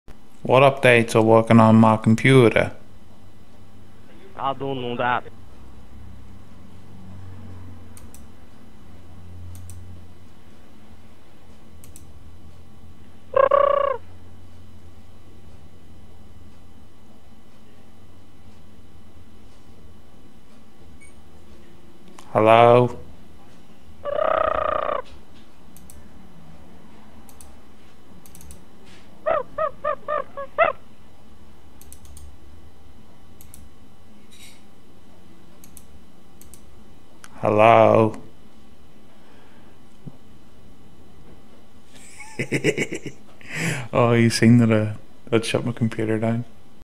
Scammer Goes Full Pigeon
This scammer decided to identify as a pigeon.